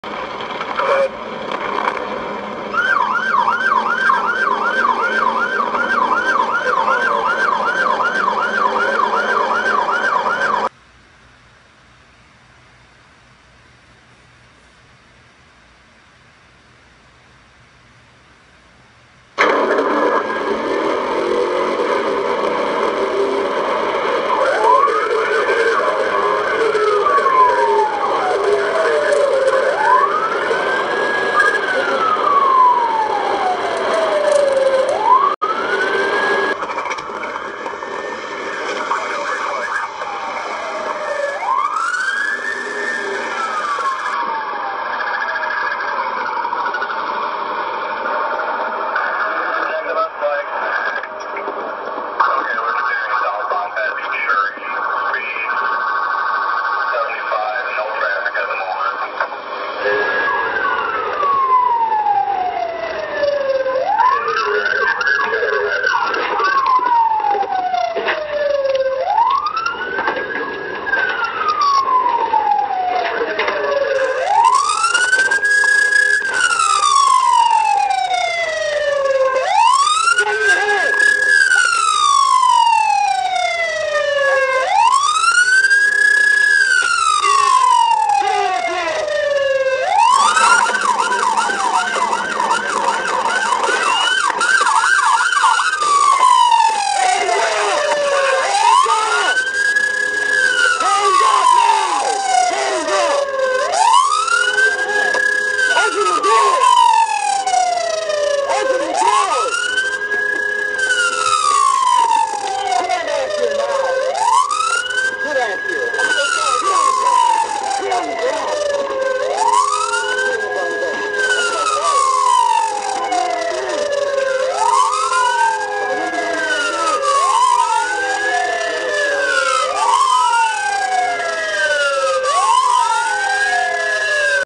FATAL: Portage Police Chase 17 Year Old, Bystander Hit and Killed
Portage MI - Potage Department of Public Safety chase David Michael Mills, a 17 year old suspect after the Kalamazoo Department of Public Safety terminated the chase.  The chase ended when the suspect hit and killed Crystal Marie Norton, a 33 year old Paw Paw, MI woman killing her.